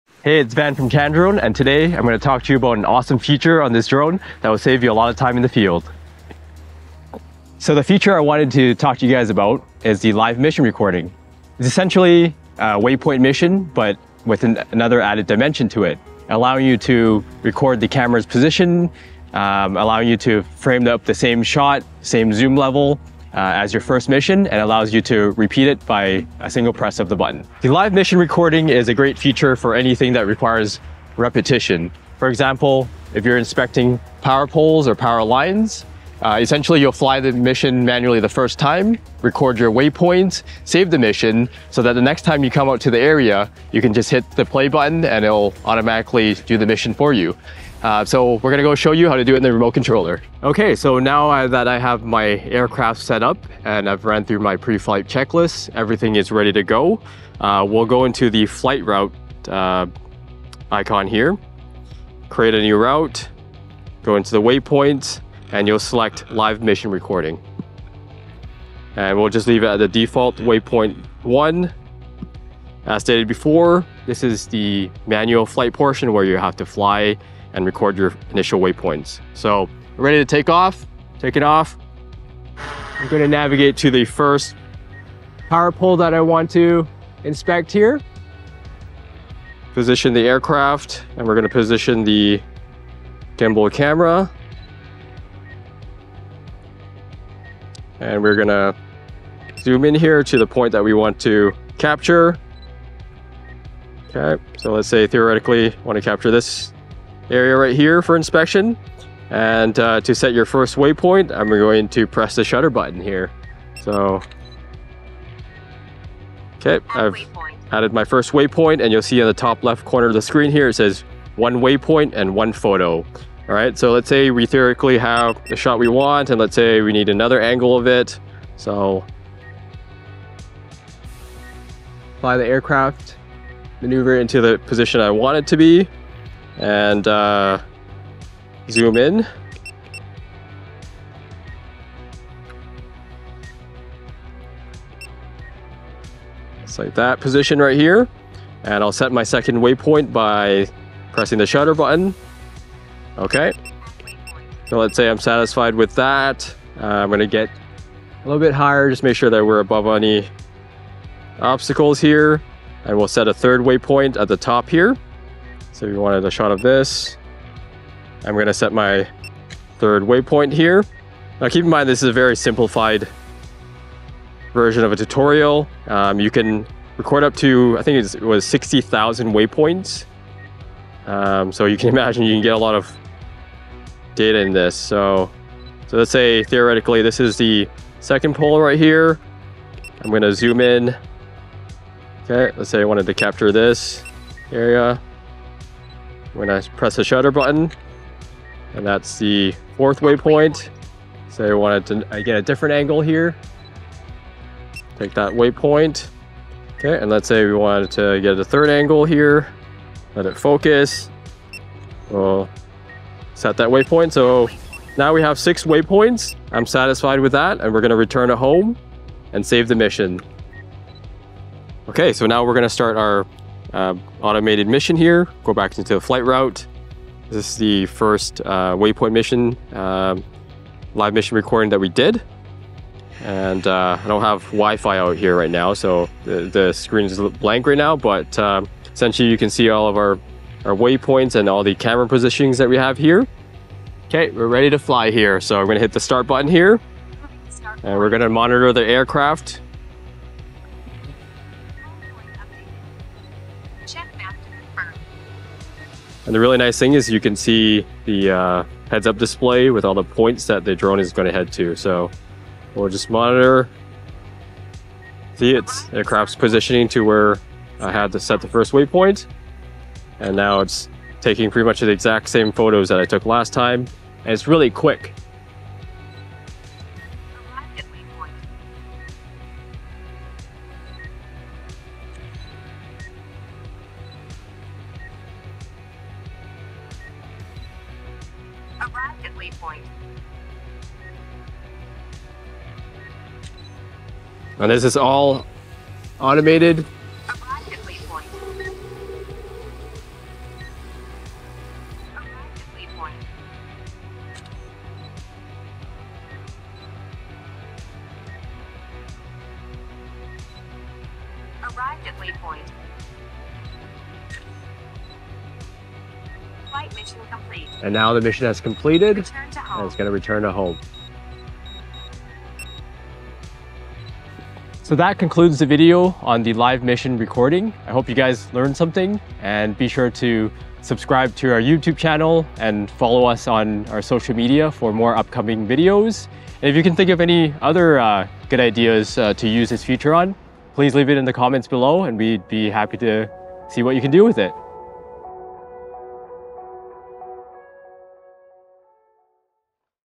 DJI M30T - Live Mission Recording
The live mission recording capabilities of DJI Enterprise series drones will save you a ton of time in the field for those repetitive tasks. We highly recommend using the D-RTK with this feature to increase the accuracy of your data.